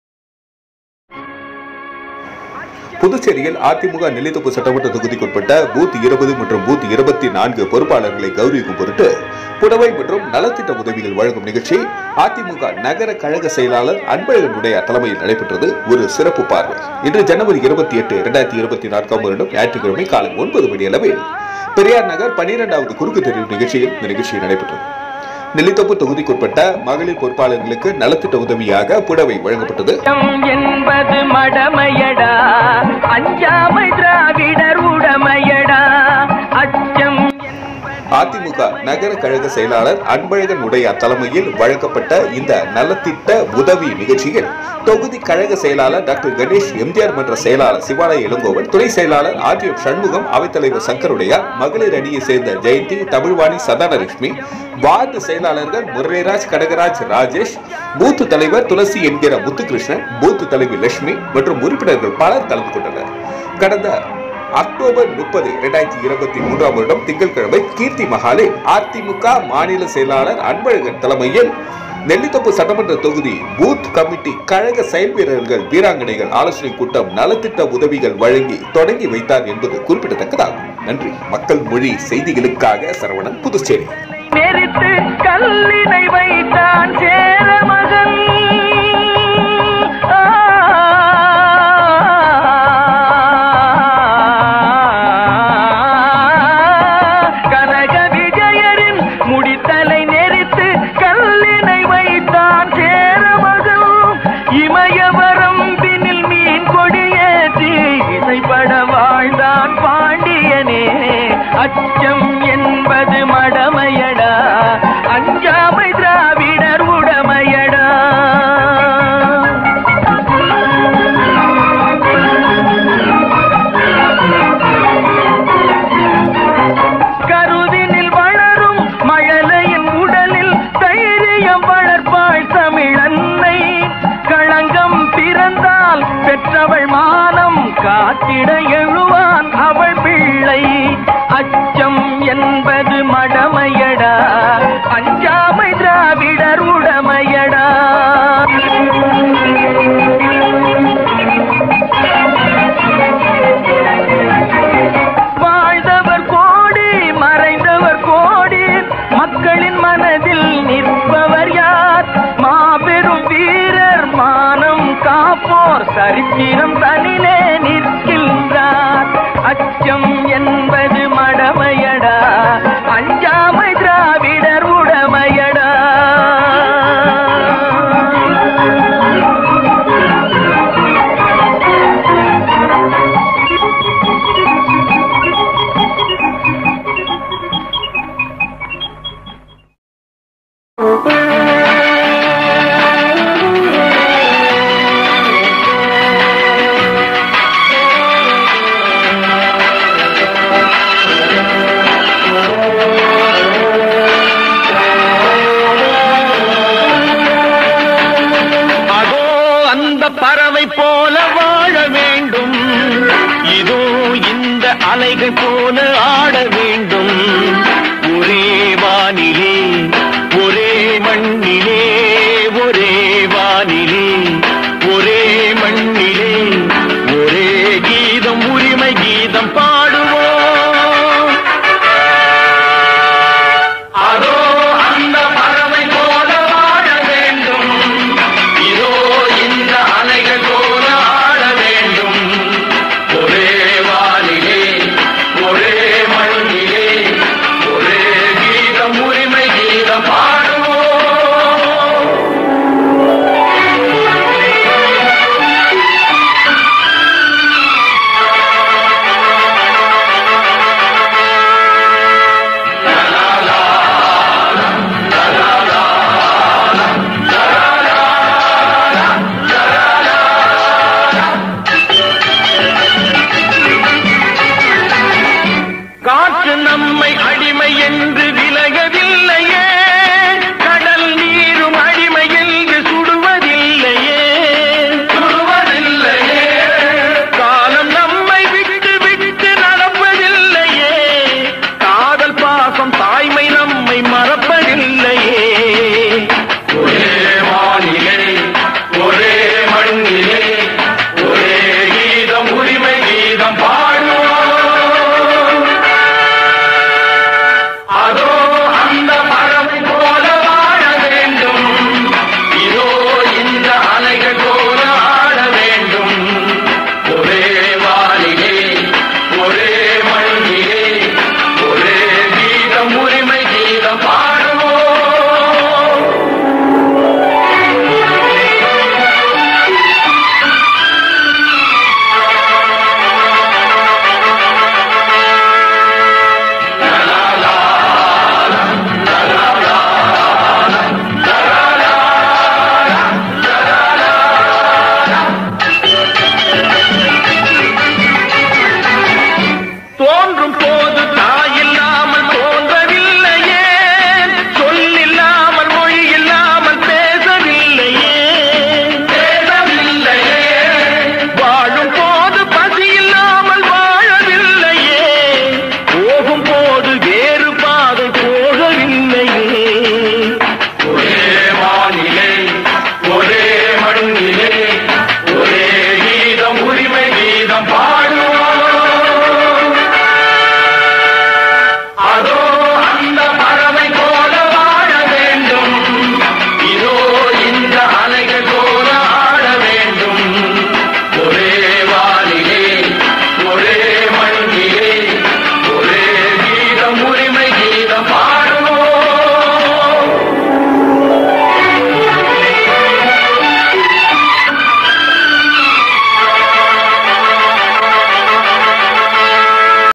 அதிமுக நெல்லித்தோப்பு சட்டமன்ற தொகுதி பூத்20 மற்றும் பூத் 24 பொறுப்பாளர்களை கௌரவிக்கும்
புதுச்சேரி:

 அதிமுக நெல்லித்தோப்பு சட்டமன்ற தொகுதி
 பூத்20 மற்றும் பூத் 24 பொறுப்பாளர்களை கௌரவிக்கும் பொருட்டு புடவை வழங்கும் நிகழ்வு
அதிமுக நகர கழக செயலாளர்
 அன்பழகன் உடையார் தலைமையில் நடைபெற்றது.

இன்று  (28-01-24) ஞாயிறு காலை 9.00 மணிக்கு 12-வது கிராஸ், பெரியார் நகர் பகுதியில் நடைபெற்றது

நெல்லித்தோப்பு தொகுதிக்குட்பட்ட  மகளிர்
பொறுப்பாளர்களுக்கு நலத்திட்ட உதவியாக புடவை வழங்கப்பட்டது.

அதிமுக நகர கழக செயலாளர் அன்பழகன் உடையார் தலைமையில் வழங்கப்பட்ட இந்த நலத்திட்ட உதவி நிகழ்ச்சியில்,

தொகுதி கழக செயலாளர் டாக்டர் கணேஷ்,
 எம்ஜிஆர் மன்ற செயலாளர் சிவாலய இளங்கோவன்,
துணைச் செயலாளர் ஆர் ஜி எம் சண்முகம், அவைத்தலைவர் சங்கர் உடையார்.

 மகளிர் அணியை சேர்ந்த ஜெயந்தி, தமிழ் வாணி சந்தானலட்சுமி

வார்டு செயலாளர்கள் முரளிராஜ், கனகராஜ், ராஜேஷ்

பூத் தலைர் துளசி @ முத்து கிருஷ்ணன்

பூத் தலைவி லட்சுமி,

மற்றும் உறுப்பினர்கள்,
மற்றும் பலர் கலந்து கொண்டனர்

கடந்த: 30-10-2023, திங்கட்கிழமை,
: கீர்த்தி மஹாலில்

அதிமுக மாநில செயலாளர் அன்பழகன் தலைமையில்
நெல்லித்தோப்பு சட்டமன்ற தொகுதி பூக் கமிட்டி கழக செயல் வீரர்கள். வீராங்கனைகள் ஆலோசனைக்
 கூட்டம் நலத்திட்ட உதவிகளை வழங்கி தொடங்கி வைத்தார் என்பது குறிப்பிடத்தக்கது.